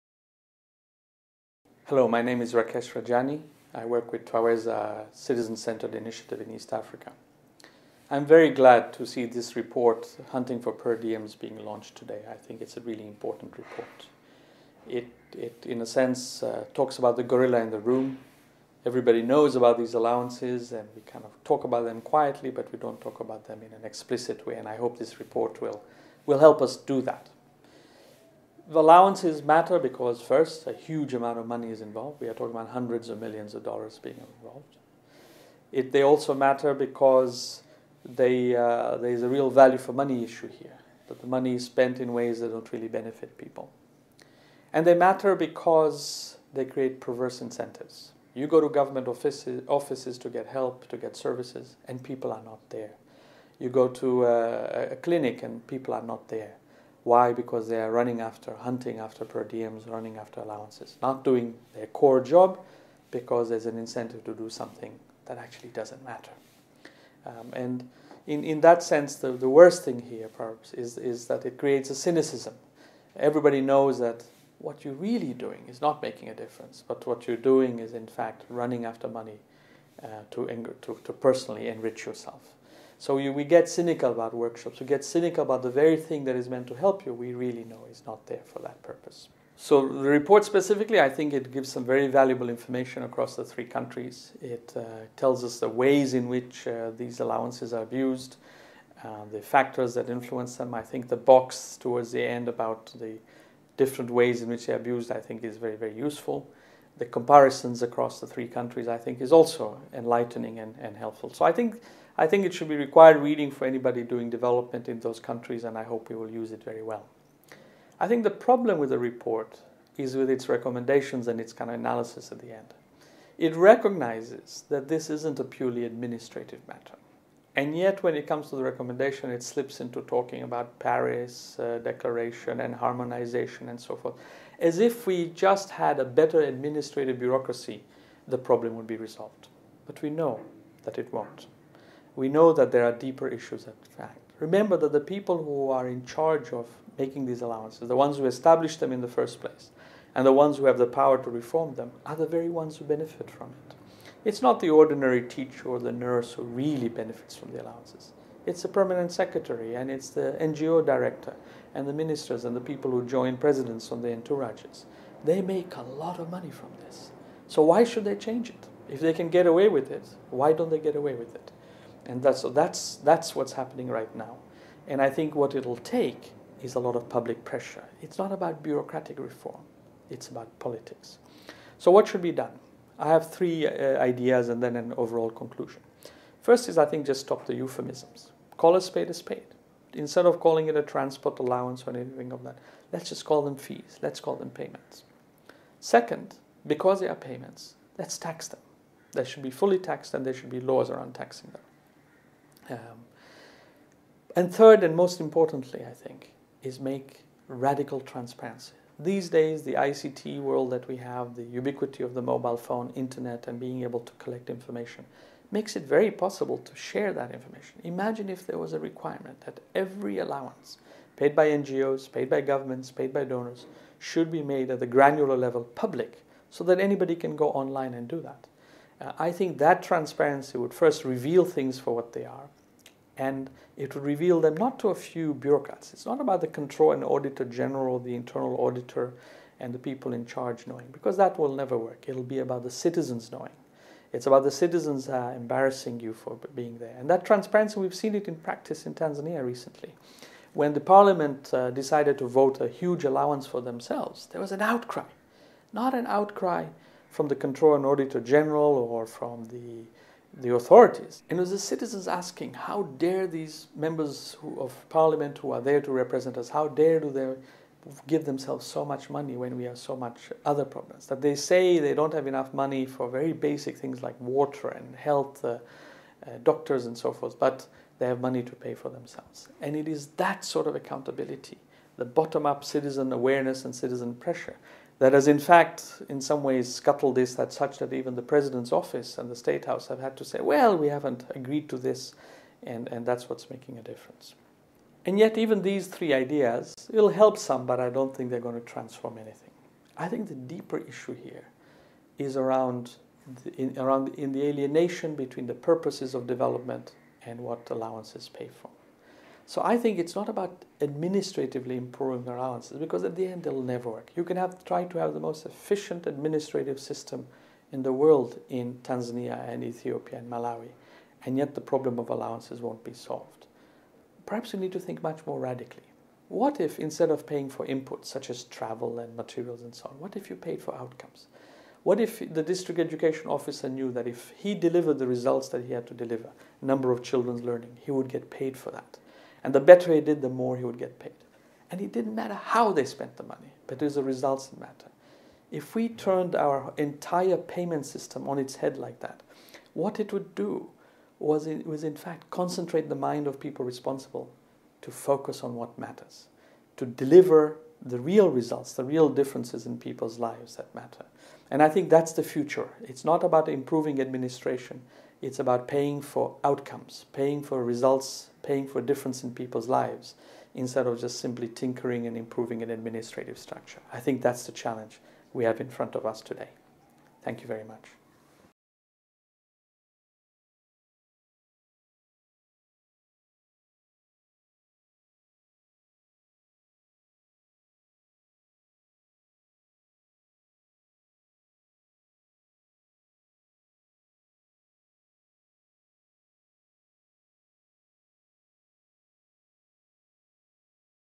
Hunting for per diem
Payment of per diems and allowances has been a matter of debate in East Africa for quite some time. A new report entitled 'Hunting for Per Diem- The Uses and Abuses of Travel Compensation in Three Developing Countries' was presented in Oslo, Norway on 23 April 2012 at a seminar organized by the Norwegian Agency for Development Cooperation (Norad). The Chr. Michelsen Insitute carried out the study in Tanzania, Malawi and Ethiopia in 2011.

In a talk given by video, Twaweza Head Rakesh Rajani finds lots of useful material in the report, including its description of the many creative forms of allowance abuse.